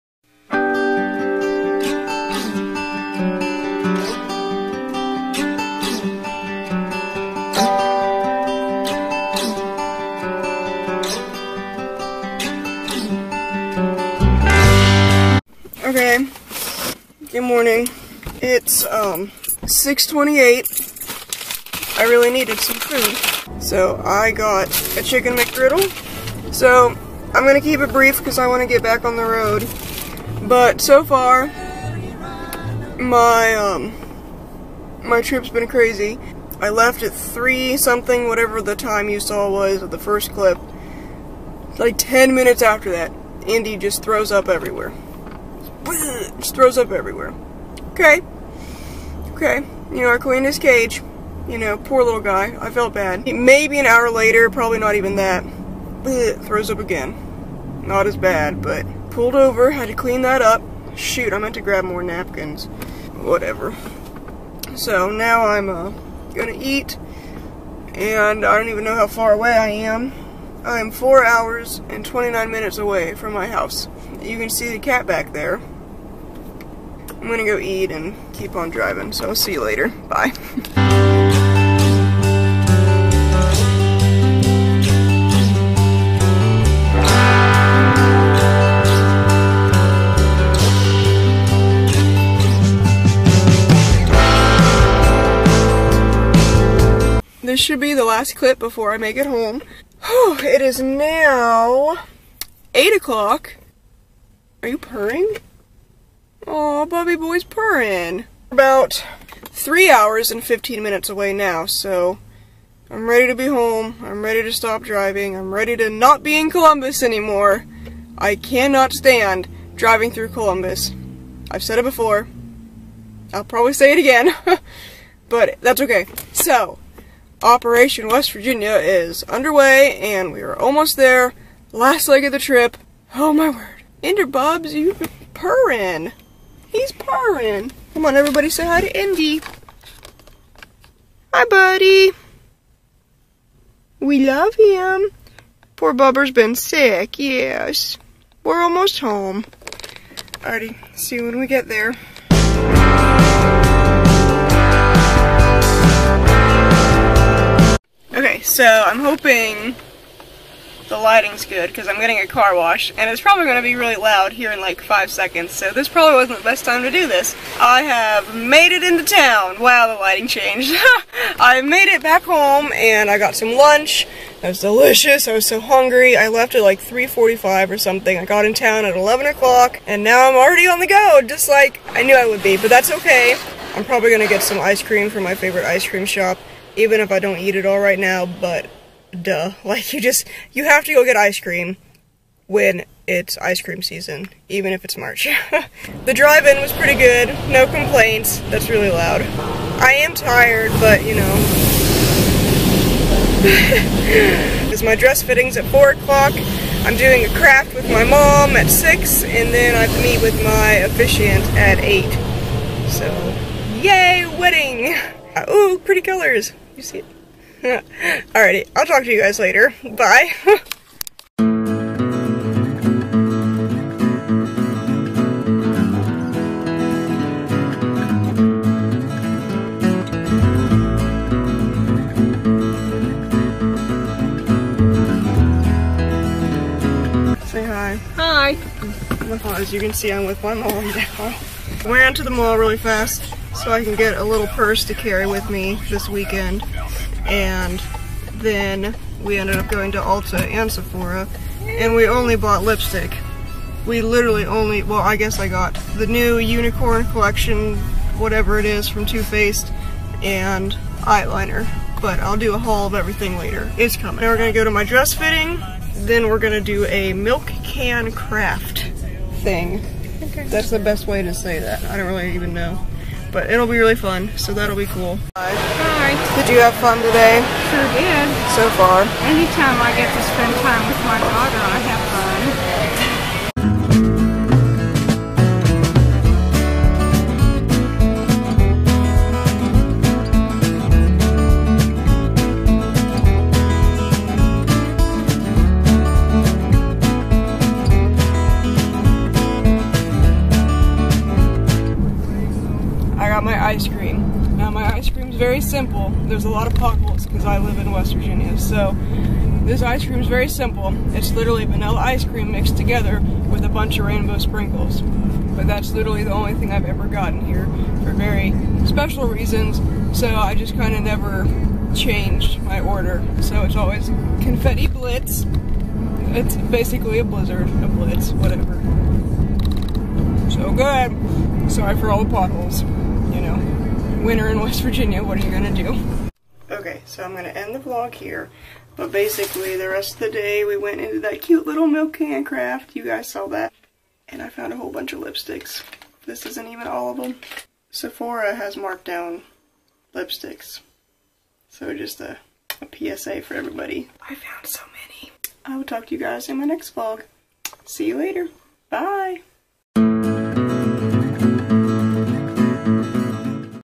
Early bird gets...to drive 7 hours💋 Seneca Red Vlogs
I'm at it again, back to West Virginia :)  Wedding planning, bachelorette things & birthday things will all be underway soon! Get ready!!

Social Media, stay in touch!